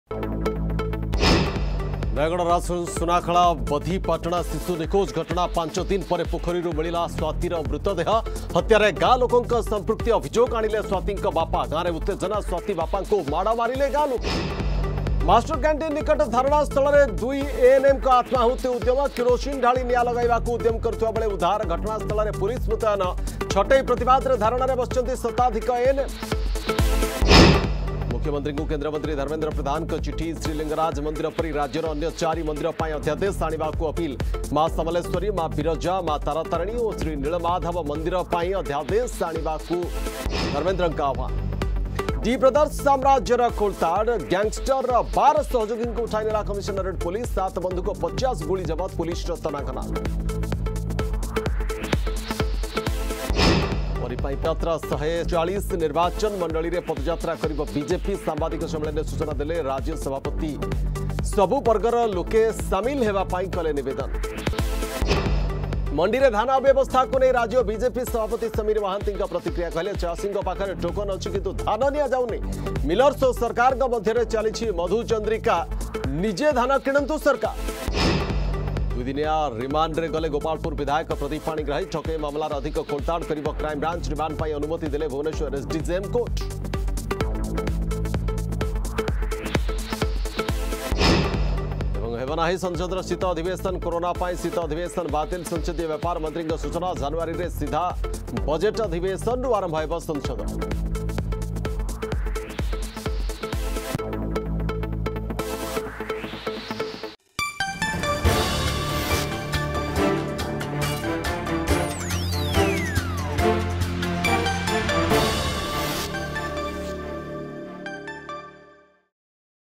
Headlines 4Pm
ନୟାଗଡ ରାଜସୁନାଖଳା ବଧିପାଟଣା ଶିଶୁ ନିଖୋଜ ଘଟଣା...
ମାଷ୍ଟରକ୍ୟାଣ୍ଟିନ ନିକଟ ଧାରଣା ସ୍ଥଳରେ 2 ANMଙ୍କ ଆତ୍ମାହୁତି ଉଦ୍ୟମ...
ମୁଖ୍ୟମନ୍ତ୍ରୀଙ୍କୁ କେନ୍ଦ୍ରମନ୍ତ୍ରୀ ଧର୍ମେନ୍ଦ୍ର ପ୍ରଧାନଙ୍କ ଚିଠି.
ଡି ବ୍ରଦର୍ସ ସାମ୍ରାଜ୍ୟର ଖୋଳତାଡ...
ପରୀ ପାଇଁ ପଦଯାତ୍ରା...
ମଣ୍ଡିରେ ଧାନ ଅବ୍ୟବସ୍ଥାକୁ ନେଇ ରାଜ୍ୟ ବିଜେପି ସଭାପତି ସମୀର ମହାନ୍ତିଙ୍କ ପ୍ରତିକ୍ରିୟା
ଦୁଇ ଦିନିଆ ରିମାଣ୍ଡରେ ଗଲେ ଗୋପାଳପୁର ବିଧାୟକ ପ୍ରଦୀପ ପାଣିଗ୍ରାହୀ...
ହେବନି ସଂସଦୀୟ ଶୀତ ଅଧିବେଶନ.